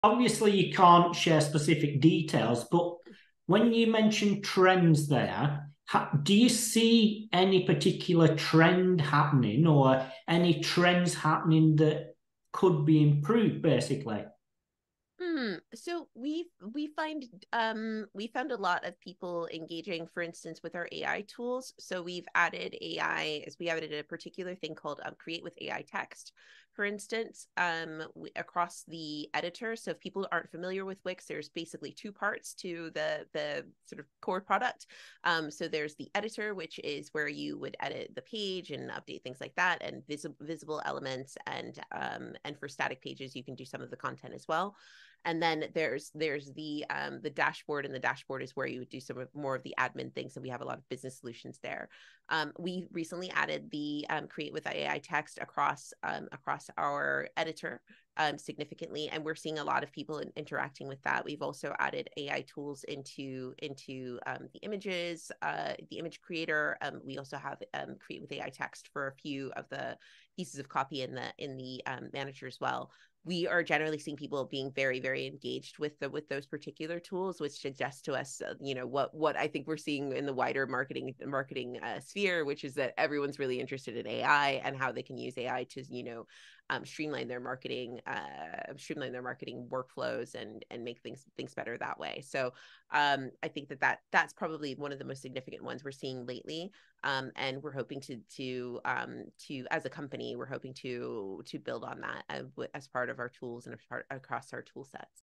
The Rising Trend and Its Impact on SEO Marketing Workflows
Join Mark A Preston and Crystal Carter as they explore the latest trend sweeping through the SEO world: the integration and rising popularity of Artificial Intelligence (AI) in optimizing marketing workflows. This video delves into the fascinating shift towards AI tools, particularly in content creation and image editing, and how businesses are increasingly leveraging AI to enhance their digital marketing strategies.

Key Insights from the Discussion:

- AI in Content Creation: Discover how AI is revolutionizing content creation, making it more efficient and accessible.
- Enhancing User Experience with AI: Learn how AI tools in platforms like Wix are streamlining the user experience in both the editor and the dashboard.
- AI's Role in Image Editing: Explore the innovative use of AI in image creation and its impact on visual content strategy.
- AI's Growing Popularity in Marketing: Understand the broader implications of AI’s popularity in digital marketing and its potential to transform traditional marketing practices.
- Future of AI in SEO and Marketing: Gain insights into how companies like Wix are planning to expand their AI toolsets to cater to evolving marketing needs.

Whether you're an SEO professional, digital marketer, or a business owner looking to stay ahead of the curve, this video provides valuable insights into the trend of AI in SEO and digital marketing. Get ready to dive into a discussion that's not just about the future of technology, but the future of effective digital marketing strategies.